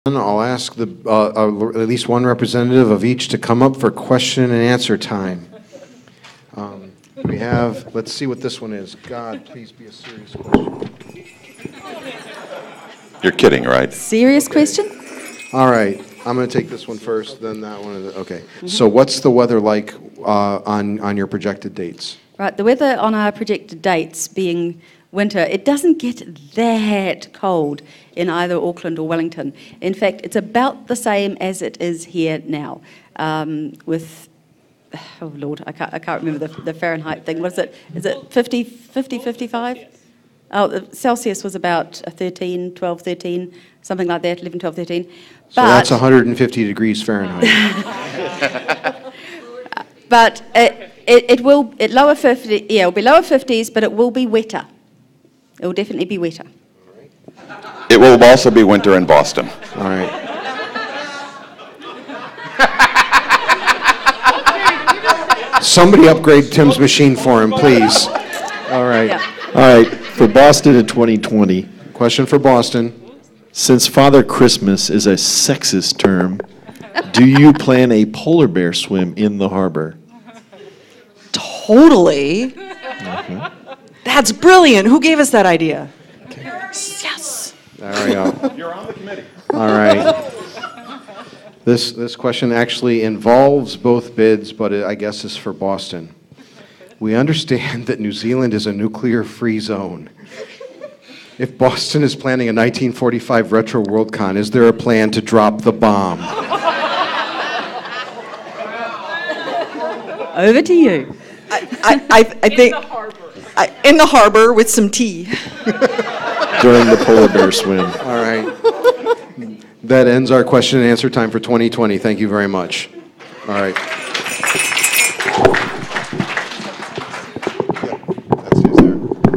Fannish Inquisition 2015: 2020 Worldcon Bid Q&A
Q&A session for the 2020 Worldcon bidders at the Fannish Inquisition at SMOFCon 33, December 5, 2015, in Fort Worth, Texas. Recorded by Lisa Hayes.